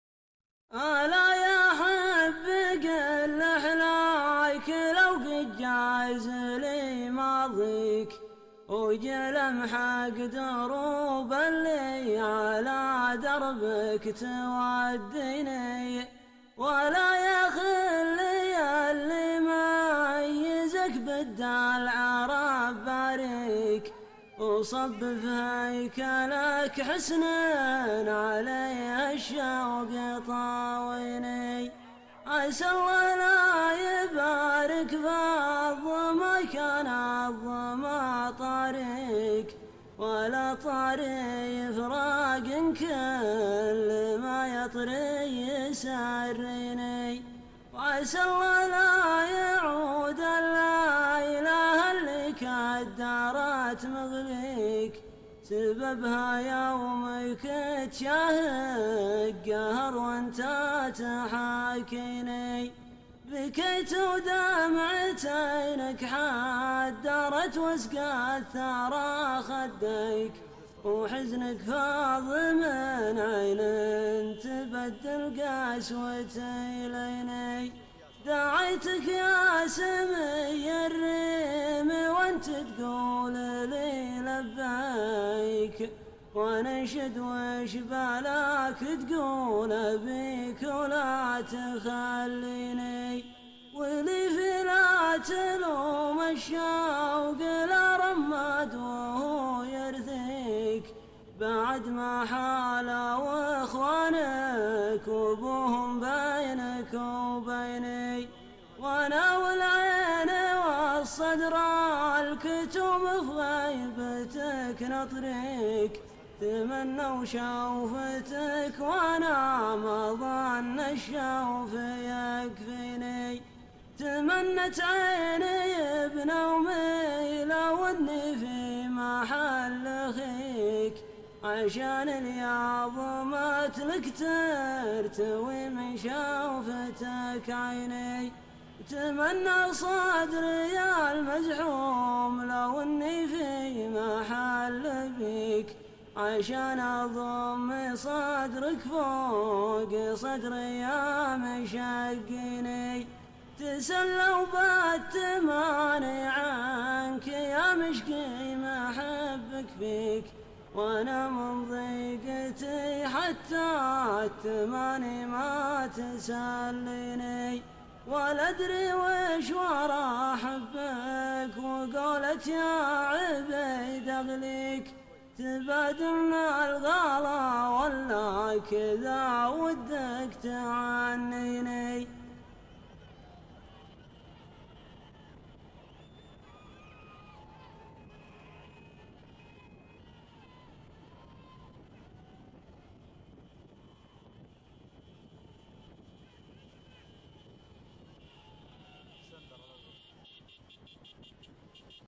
هدد اكسندر
هدد سميحان العرماني بتاريخ 1433/12/15هـ   وفوز السلوقي اكسندر مسافة 2500 م